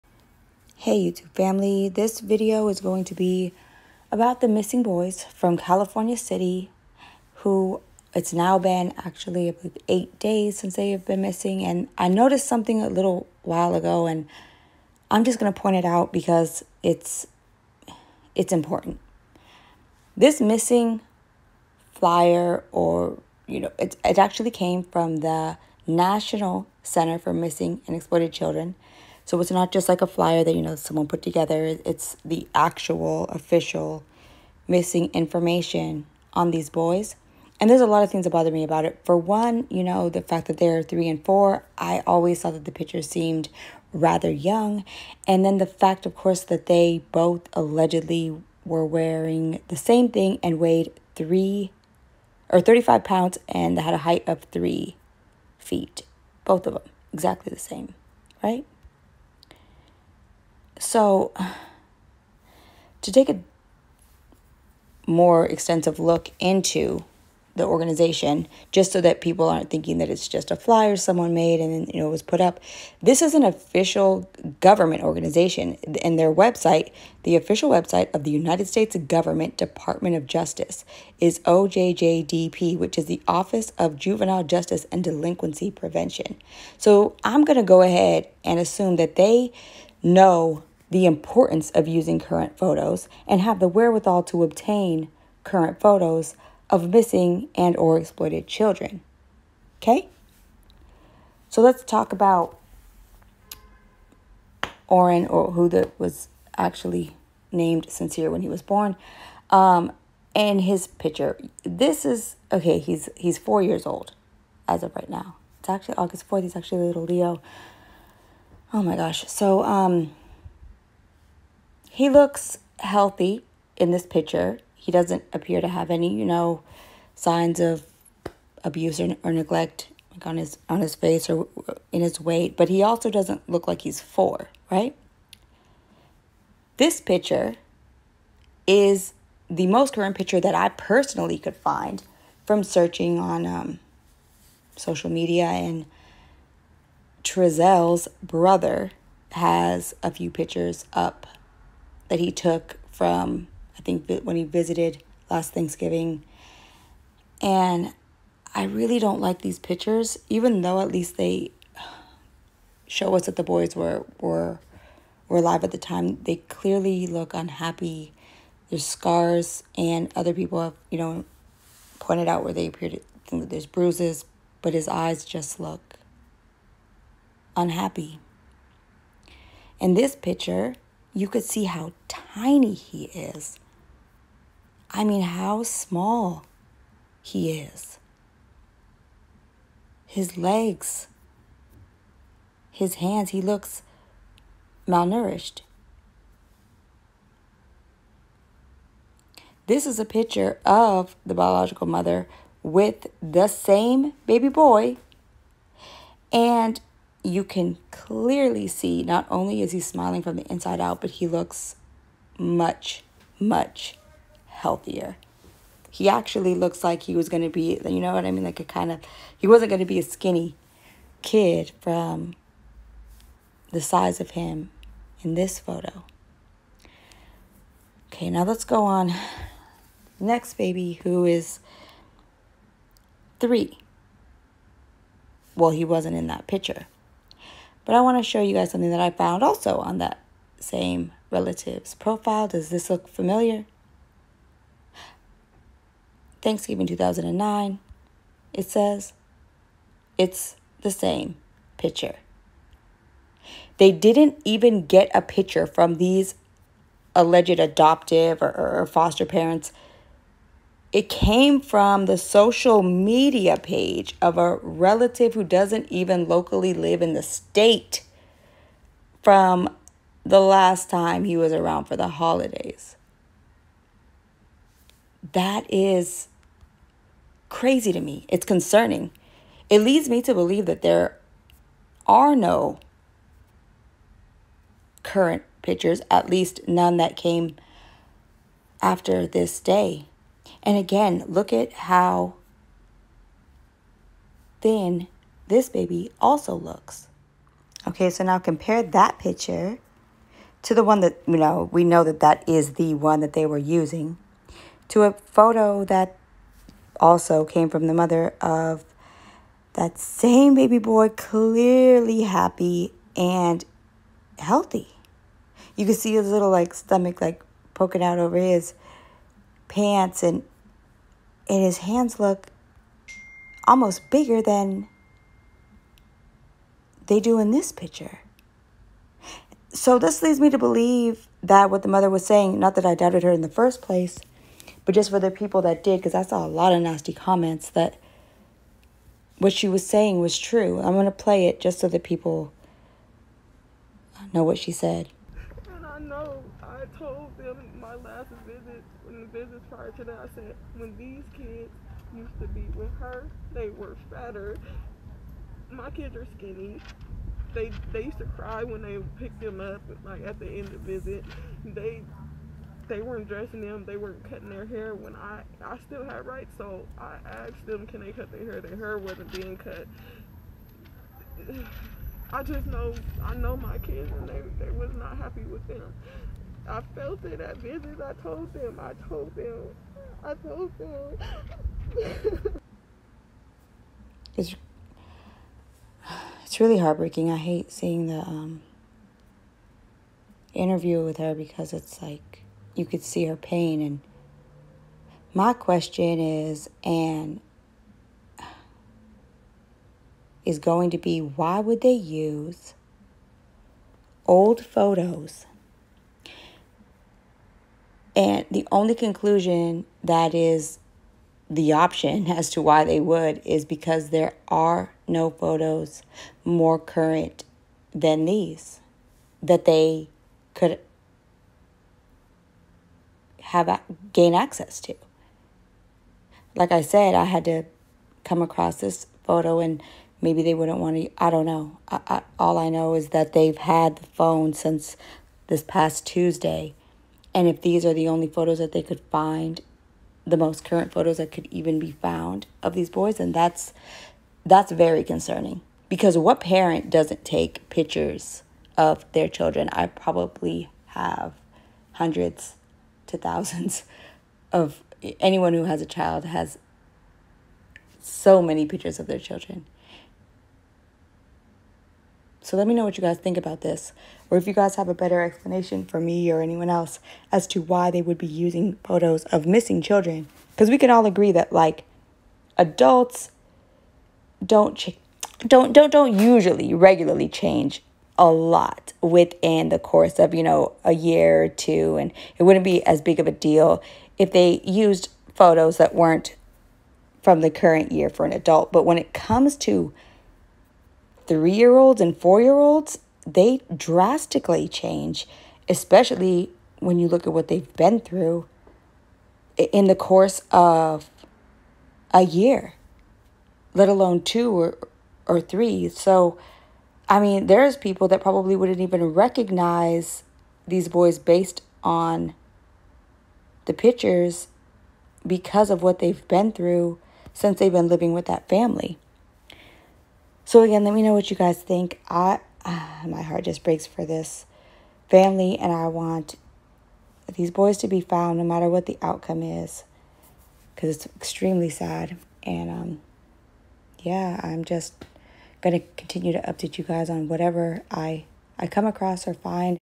Why don’t Jacqueline & Trezell West have current photos of the boys missing from California City??
Typically when children go missing law enforcement releases the MOST CURRENT photos, height and weight available to the public. We all know that features and attributes can change drastically from year to year as children grow. So why would they use such outdated photos???